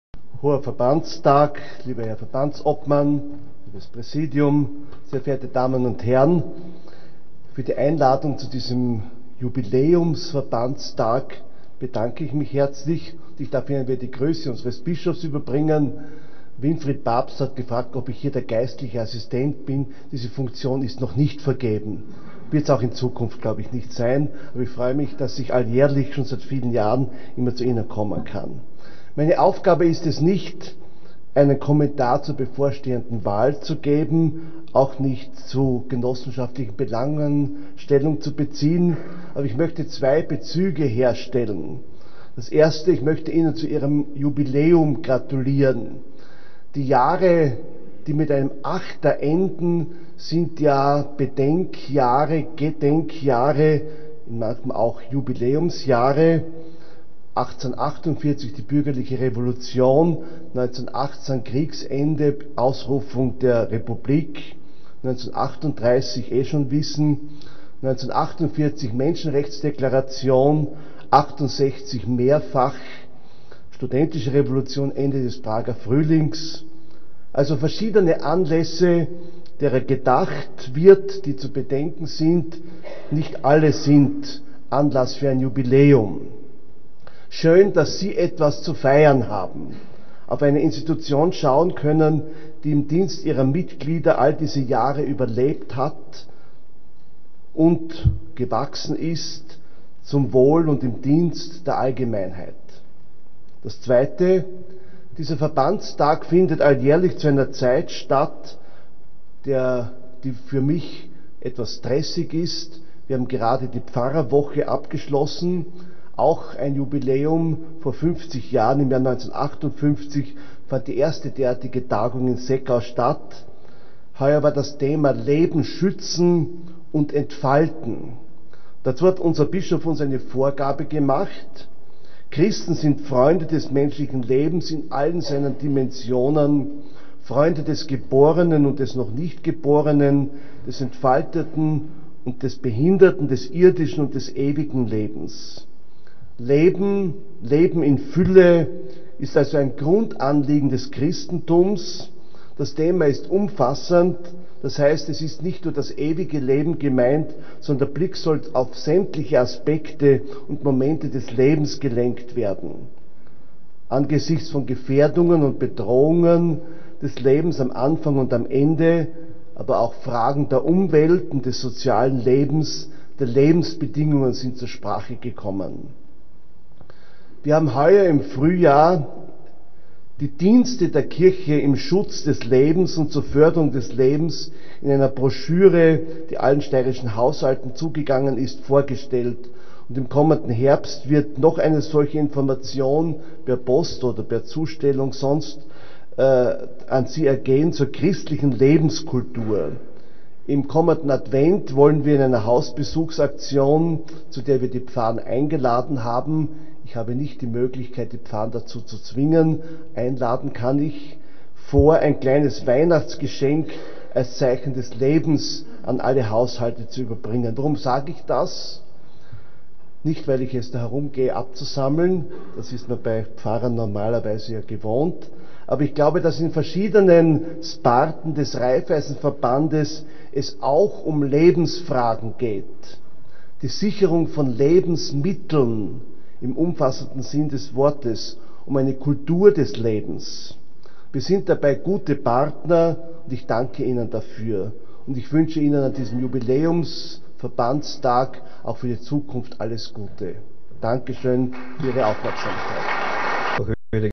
Verbandstag 2008: Grußworte Bischofsvikar Dr. Heinrich Schnuderl
65. Verbandstag des Raiffeisenverbandes Steiermark
26. September 2008, Messe Congress Graz
Motto: "70 Jahre Raiffeisenverband Steiermark"

Grußworte von Bischofsvikar Dr. Heinrich Schnuderl